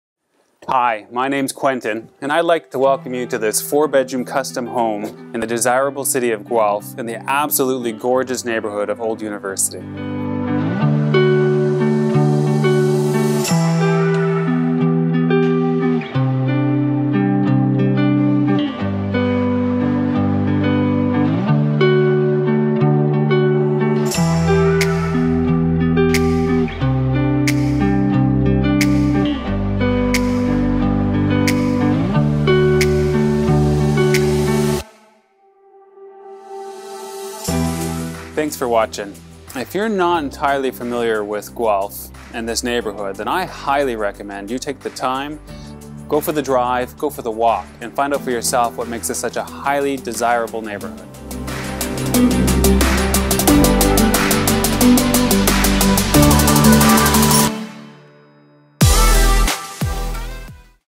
TW/TV I 61 Dean Ave
Welcome to this Old University Village 4 bedroom, custom-built and luxurious home. You will be hard-pressed to find a more beautiful neighborhood in Guelph - walkable to downtown, Stone Rd., trails, parks, the River, and of course, the University of Guelph. The home's exterior of stone, wood, and the wrap-around porch is a tasteful nod to a simpler time. Inside; a kitchen designed for families, entertaining, and exploring your cooking interests. Quartz countertops, 5-foot wide island slab, high-end Kitchenaid appliance package including a commercial grade 6 burner 36" wide stove. Floating shelves, tasteful lighting, charcoal backsplash, and plenty of storage all off-set with the warm contrast of the Cafe' Noir Oak floors. Built-in speaker system. Wired with CAT5 and ready for your internet demands. Living room with "heart and glow" gas fireplace (sorry, the fireplace mantel was installed post photos, book a showing to check it out!), double door walk-out to the porch and a spacious backyard. A two-piece powder room off the two-car garage is graced with a mudroom sink and bench/cubby area. (I did say this home was ideal for families, didn't I?). Head upstairs and you will find each bedroom has a substantial, walk-in size closet. Large main bath with double sinks. 2nd-floor laundry. A master retreat "wing" that was designed to spoil - including the walk-in closet with space for his and hers. The ensuite features all the right pieces; glass walk-in shower, high-end fixtures, a stand-alone tub, double sinks, more quartz and the beauty of generous natural light - it is a true retreat. The basement is roughed-in for an accessory apartment and has a separate entrance, loads of light, and a large window for egress. So, the question is, are you ready to move to the perfect home in the perfect neighbourhood?